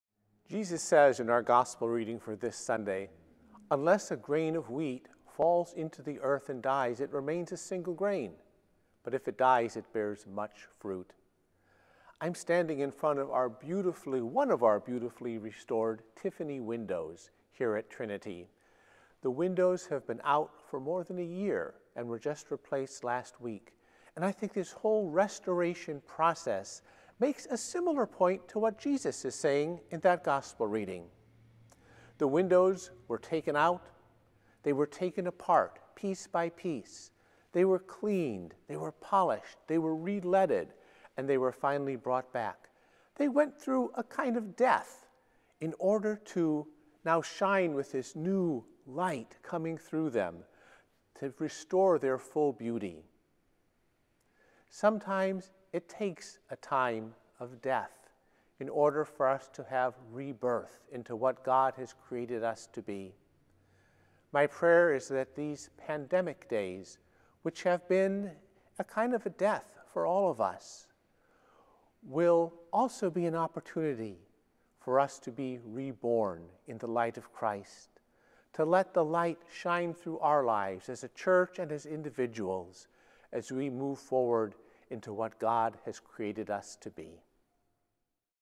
Restored Windows
Pastor Alan discovers a connection between our restored Tiffany windows and the buried seed in next Sunday’s Gospel.